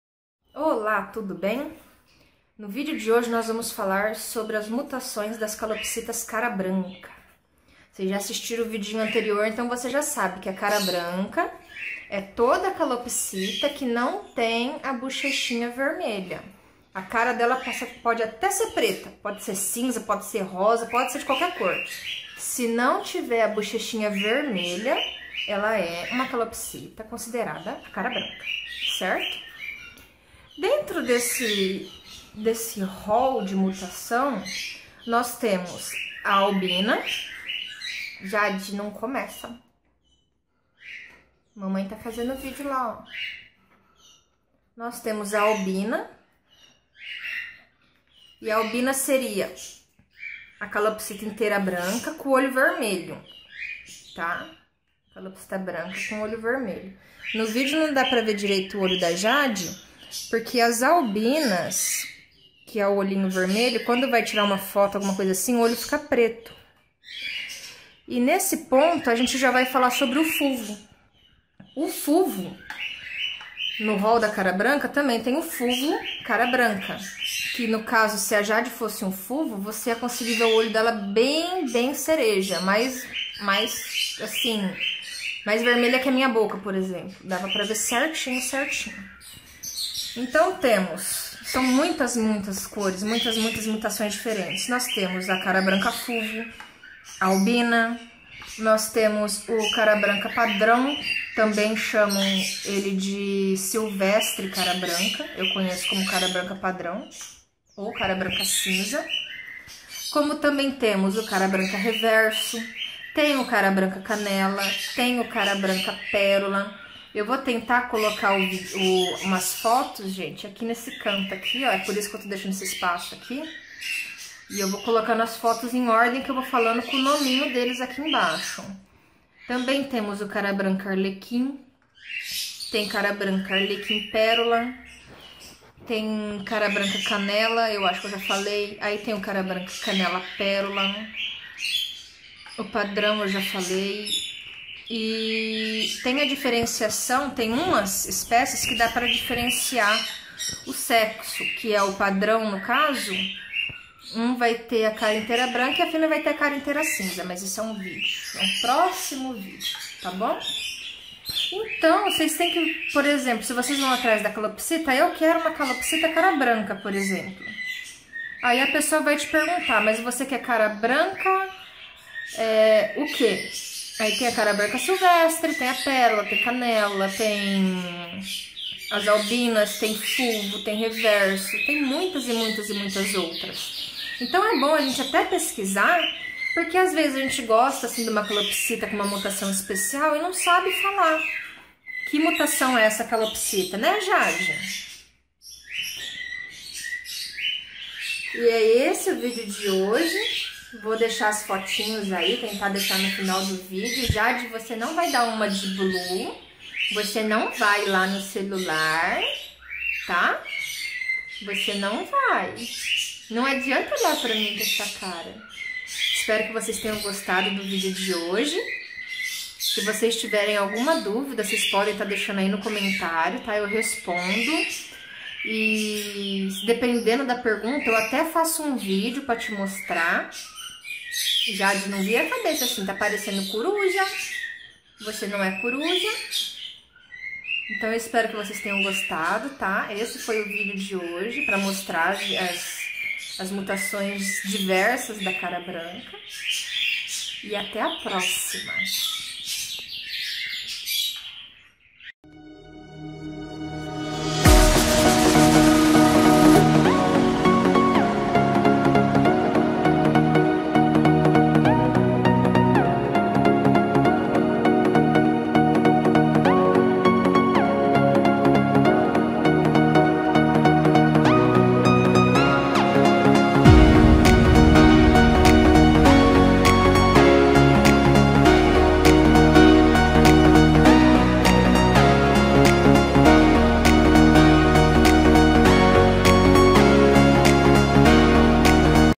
Mutação de Calopsita - Cara Branca parte 2
No vídeo de hoje vou falar um pouco sobre as diversas mutações que a calopsita cara branca tem.